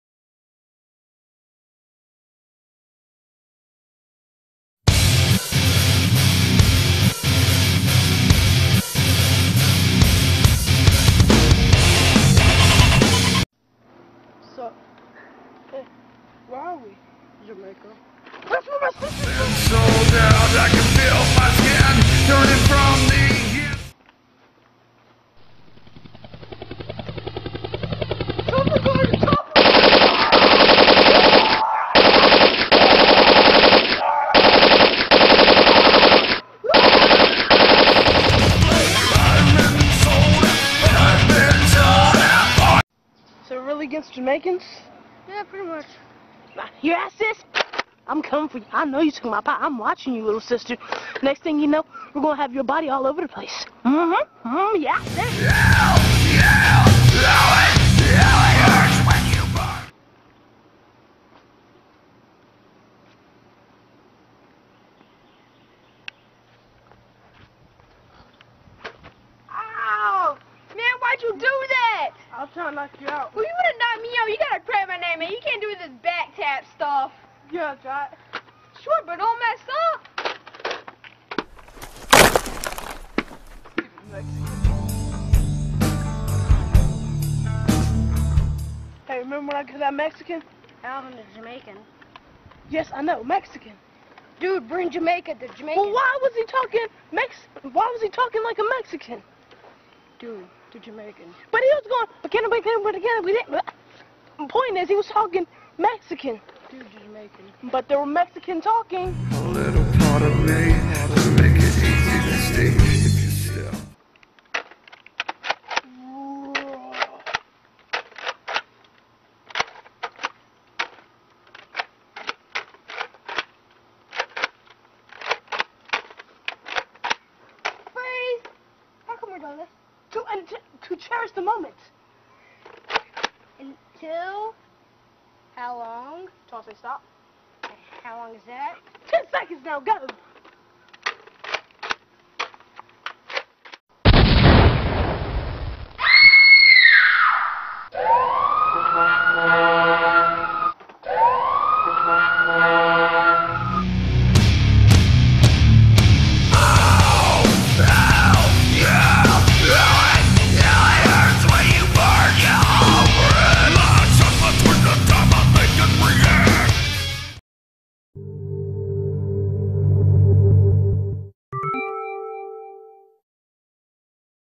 Saving Private P.I.E * trailer *
2 retarded marines in the secret Ops called P.I.E or ( pie i eat ) come to Jamaica in search of their pie. while their stay they act goofy and stupid and will most likely make you laugh.